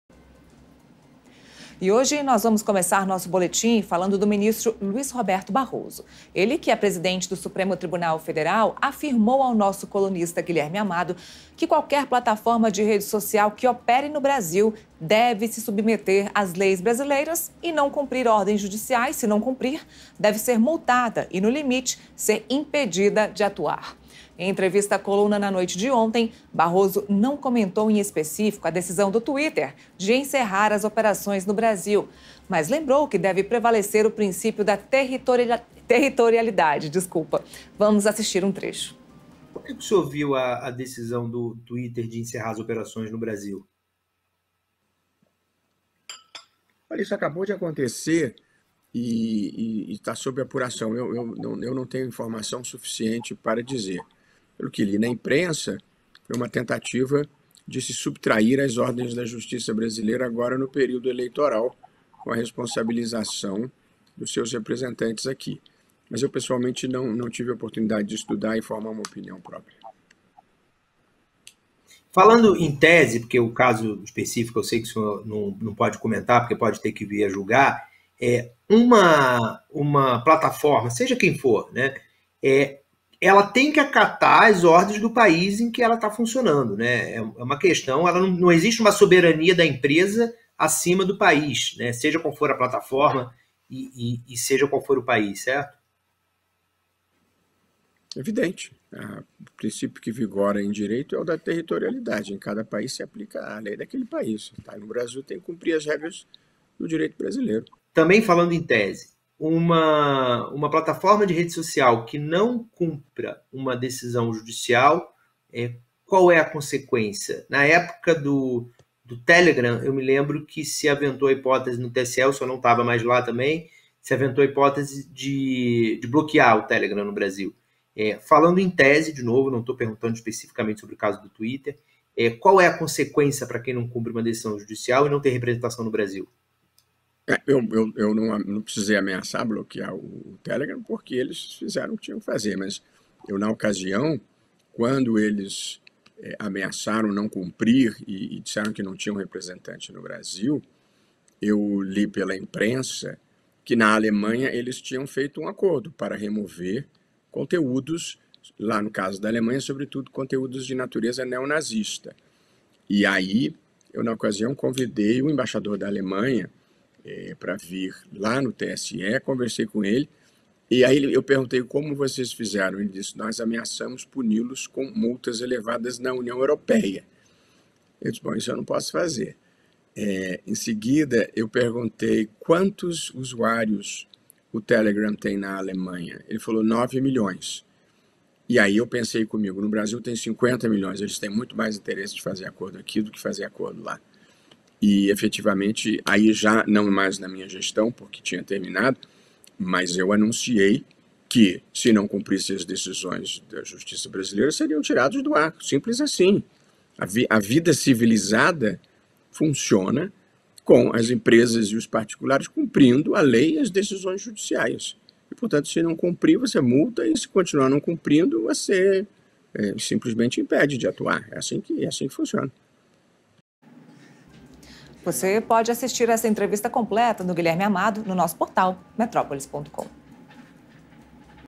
Barroso afirma que qualquer rede social no Brasil deve se submeter às leis brasileiras
O ministro Luís Roberto Barroso, presidente do STF, afirmou que qualquer plataforma de rede social em operação no Brasil deve se submeter às leis brasileiras. Caso não cumpra as ordens judiciais, deve ser multada e, no limite, impedida de atuar.

Em entrevista à coluna, na noite desse domingo, Barroso não comentou em específico a decisão do Twitter de encerrar as operações no Brasil, mas lembrou que deve prevalecer o princípio da territorialidade. Assista aqui à íntegra da entrevista.